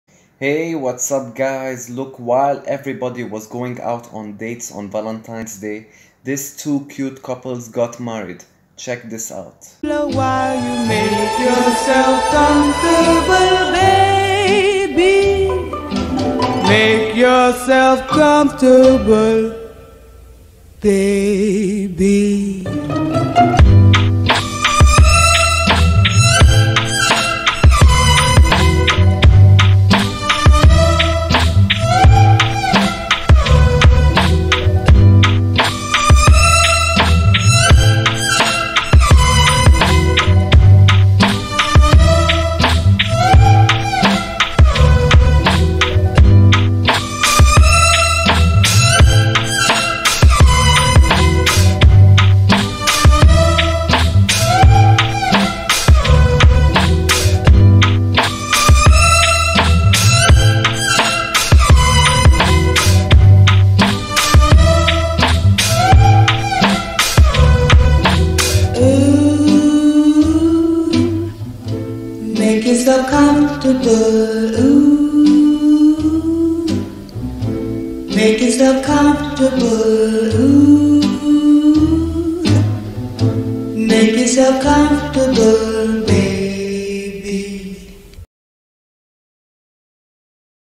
Cat wedding, Cute Cats get married!
Hilarious Cat Wedding!
While everyone was going on Dates on Valentines day, these two cute cats got married